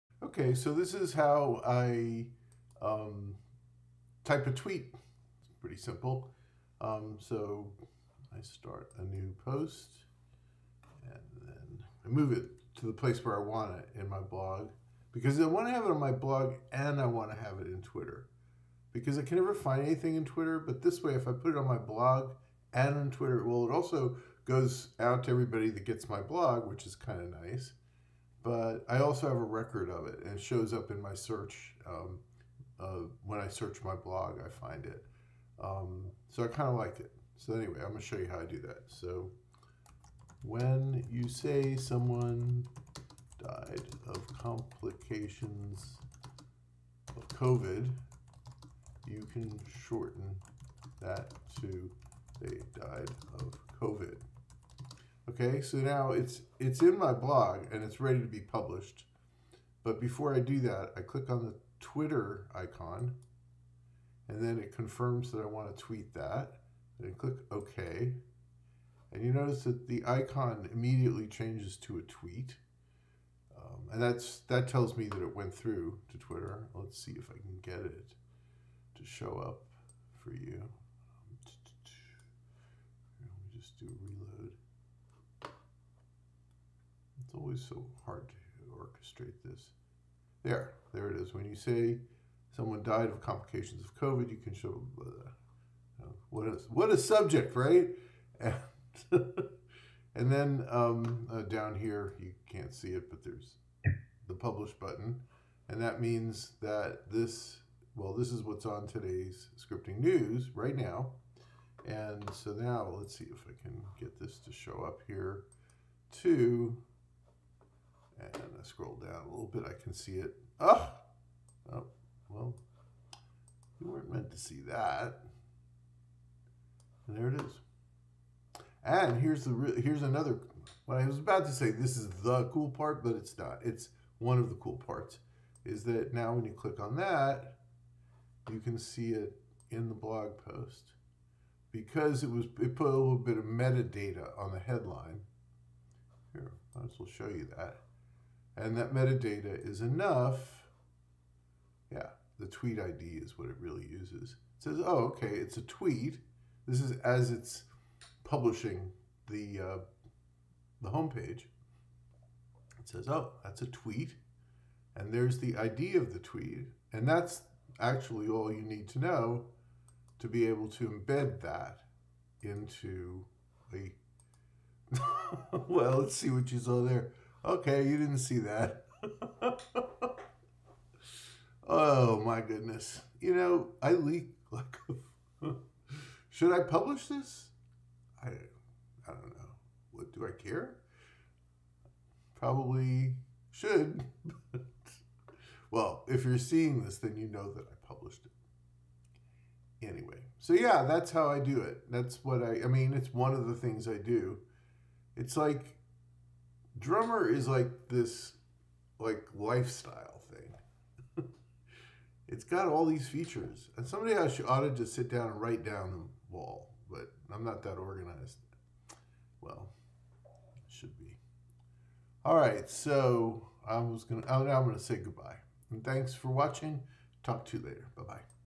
How I tweet in my outliner so it's also published on my blog
Lots of synergy between the two.